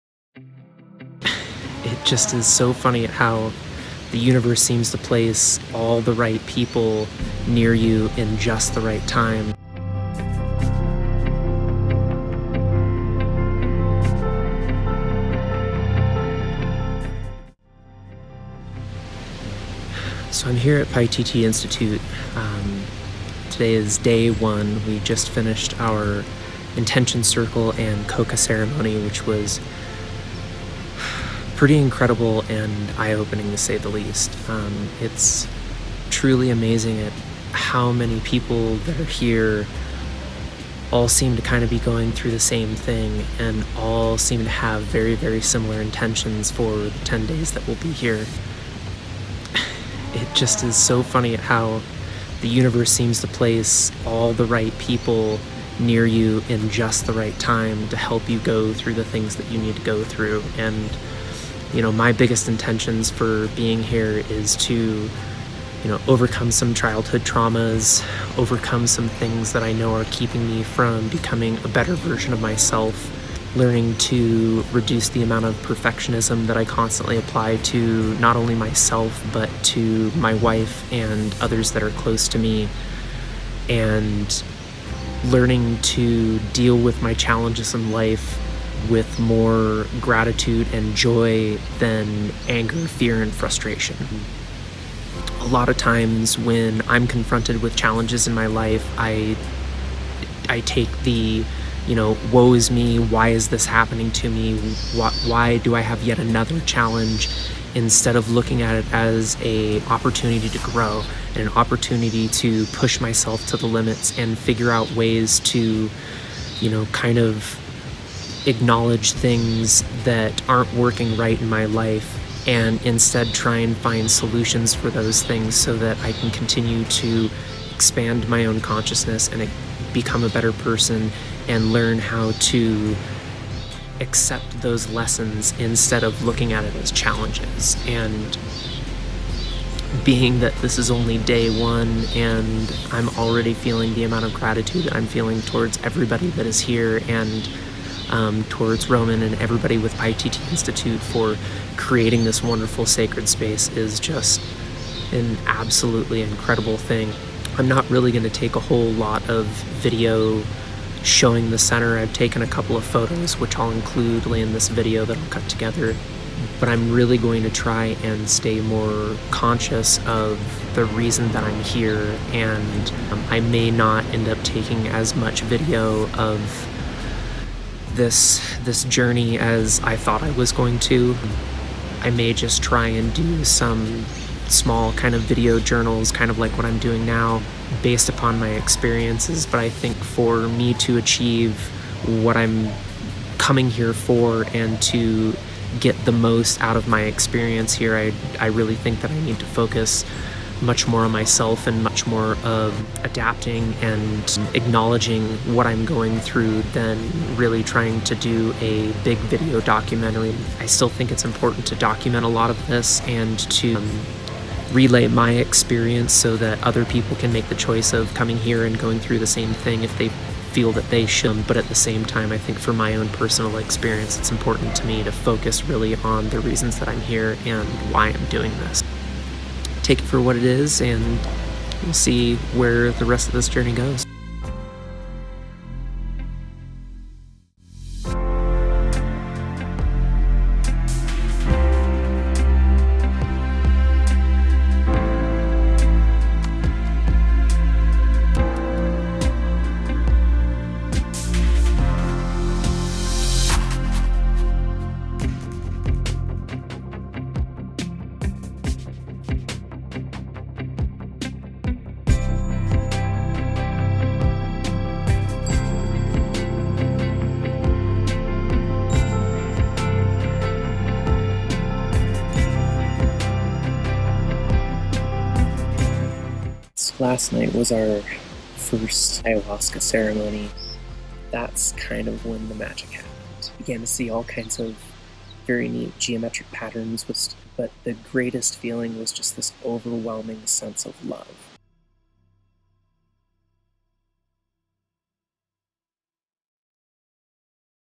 Traveling to Peru - Day 4 - Arriving at Paititi Institute
In this episode, I detail my experience with arriving at Paititi Institute and my thoughts, after our intention circle and coca ceremony, in preparation for our first ayahuasca ceremony and the rest of the 10-day retreat.

Tripod: Manfroto Mini Tripod with Smartphone mount

Microphone: Rode SmartLav+